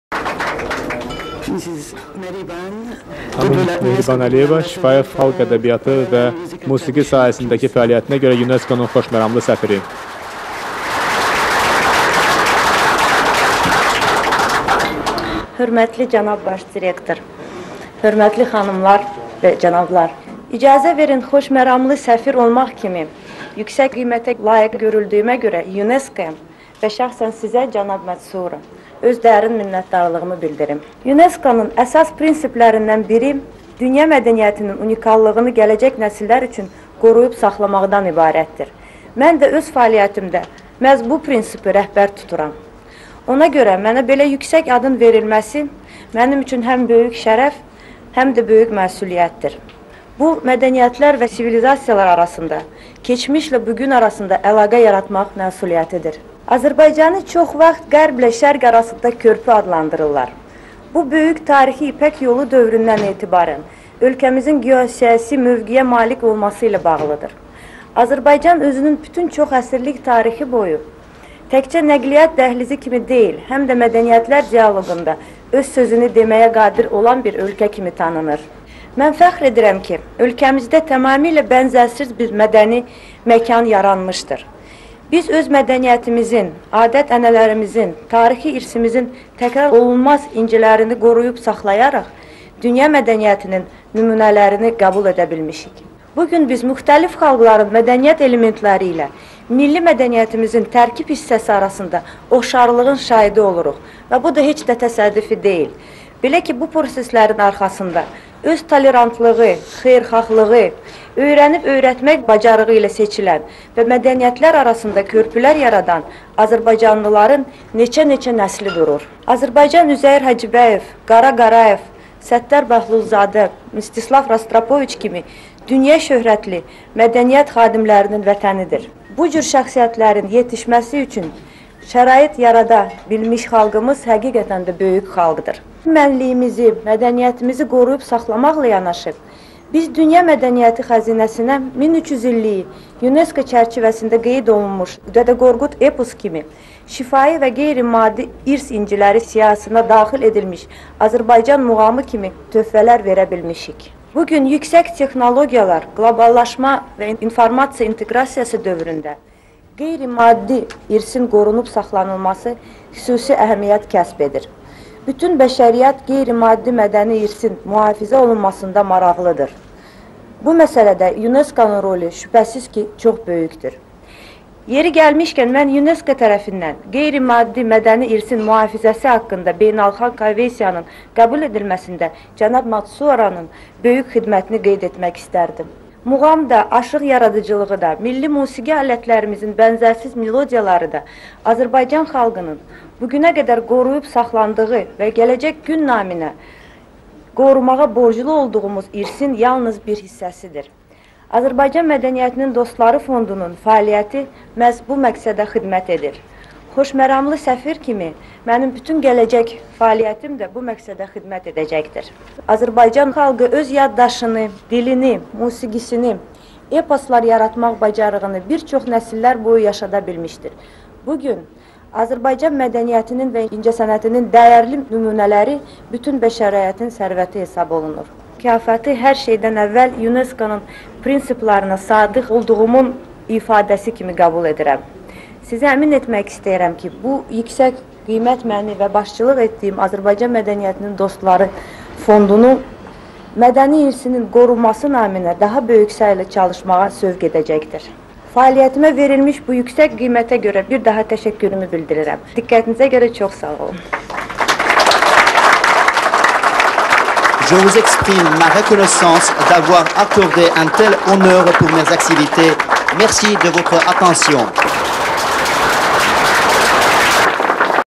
Mehriban Əliyeva - Xoşməramlı səfir
9 sentyabr 2004

Mehriban Əliyevaya UNESCO-nun Xoşməramlı səfiri adı verilməsinə həsr olunmuş mərasim

Sentyabrın 9-da Parisdə, UNESCO-nun iqamətgahında Azərbaycanın birinci xanımı Mehriban Əliyevaya UNESCO-nun Xoşməramlı səfiri adı verilməsinə həsr olunmuş mərasim keçirilib.

Birinci xanım Mehriban Əliyeva mərasimdə çıxış edib.

Mehriban Əliyevanın çıxışı:

- Hörmətli cənab Baş direktor!

Hörmətli xanımlar və cənablar!

İcazə verin, Xoşməramlı səfir kimi yüksək ada layiq görüldüyümə görə YUNESKO-ya və cənab Matsuura, şəxsən Sizə, öz dərin minnətdarlığımı bildirim.

YUNESKO-nun əsas prinsiplərindən biri dünya mədəniyyətinin nadirliyini gələcək nəsillər üçün qoruyub saxlamaqdan ibarətdir. Mən də öz fəaliyyətimdə bu prinsipi əsas tuturam. Ona görə mənə bu yüksək adın verilməsi mənim üçün həm böyük şərəf, həm də böyük məsuliyyətdir. Bu, mədəniyyətlər və sivilizasiyalar arasında, keçmişlə bu gün arasında əlaqə yaratmaq məsuliyyətidir.

Azərbaycanı çox vaxt Qərblə Şərq arasında körpü adlandırırlar. Bu, hələ tarixi Böyük İpək yolu dövründən ölkəmizin əlverişli coğrafi-siyasi mövqeyə malik olması ilə bağlıdır. Azərbaycan özünün çoxəsrlik tarixi boyunca təkcə nəqliyyat dəhlizi kimi deyil, həm də mədəniyyətlərin dialoqunda öz sözünü deməyə qadir olan bir ölkə kimi tanınır. Mən fəxr edirəm ki, ölkəmizdə tamamilə bənzərsiz bir mədəni məkan yaranmışdır. Biz öz mədəniyyətimizin, adət-ənənələrimizin, tarixi irsimizin təkrarolunmaz incilərini qoruyub saxlayaraq, dünya mədəniyyətinin nümunələrini qəbul edə bilmişik.

Bu gün biz müxtəlif xalqların mədəniyyət elementləri ilə milli mədəniyyətimizin tərkib hissəsi arasında oxşarlığın şahidi oluruq. Bu isə heç də təsadüfi deyildir. Belə ki, bu proseslərin arxasında öz tolerantlığı, xeyirxahlığı, öyrənib-öyrətmək bacarığı ilə seçilən və mədəniyyətlər arasında körpülər yaradan azərbaycanlıların neçə-neçə nəsli durur.

Azərbaycan Üzeyir Hacıbəyov, Qara Qarayev, Səttar Bəhlulzadə, Mstislav Rostropoviç kimi dünya şöhrətli mədəniyyət xadimlərinin vətənidir. Bu cür şəxsiyyətlərin yetişməsi üçün şərait yarada bilmiş xalqımız, həqiqətən də, böyük xalqdır.

Öz milli mənliyimizi, mədəniyyətimizi qoruyub saxlamaqla yanaşı, biz dünya mədəniyyəti xəzinəsinə 1300 illiyi YUNESKO çərçivəsində qeyd olunmuş "Kitabi-Dədə Qorqud" eposu, şifahi və qeyri-maddi irs inciləri siyahısına daxil edilmiş Azərbaycan muğamı kimi töhfələr verə bilmişik. Bu gün, yüksək texnologiyalar, qloballaşma və informasiya inteqrasiyası dövründə qeyri-maddi irsin qorunub saxlanılması xüsusi əhəmiyyət kəsb edir. Bütün bəşəriyyət qeyri-maddi irsin qorunmasında çox maraqlıdır. Bu məsələdə YUNESKO-nun rolu, şübhəsiz ki, çox böyükdür.

Yeri gəlmişkən, mən YUNESKO tərəfindən qeyri-maddi irsin qorunması haqqında Konvensiyanın qəbul edilməsində cənab Matsuuranın böyük xidmətini vurğulamaq istərdim.

Muğam da, aşıq yaradıcılığı da, milli musiqi alətlərimizin bənzərsiz melodiyaları da Azərbaycan xalqının bu günə qədər qoruyub saxladığı və gələcək gün naminə qorumağa borclu olduğumuz irsin yalnız bir hissəsidir. Azərbaycan Mədəniyyətinin Dostları Fondunun fəaliyyəti məhz bu məqsədə xidmət edir. Xoşməramlı səfir kimi mənim bütün gələcək fəaliyyətim də bu məqsədə xidmət edəcəkdir.

Azərbaycan xalqı öz yaddaşını, dilini, musiqisini, eposlar yaratmaq bacarığını bir çox nəsillər boyu yaşada bilmişdir. Bu gün Azərbaycan mədəniyyətinin və incəsənətinin dəyərli nümunələri bütün bəşəriyyətin sərvəti hesab olunur.

Mənə bu gün verilən fəxri adı, hər şeydən əvvəl, YUNESKO-nun prinsiplərinə sadiq olmağımın ifadəsi kimi qəbul edirəm.

Sizi əmin etmək istəyirəm ki, bu yüksək mükafat məni və başçılıq etdiyim Azərbaycan Mədəniyyətinin Dostları Fondunu mədəni irsimizin qorunması naminə daha böyük səylə çalışmağa sövq edəcəkdir.

Fəaliyyətimə verilmiş bu yüksək qiymətə görə bir daha təşəkkürümü bildirirəm.

Diqqətinizə görə sağ olun.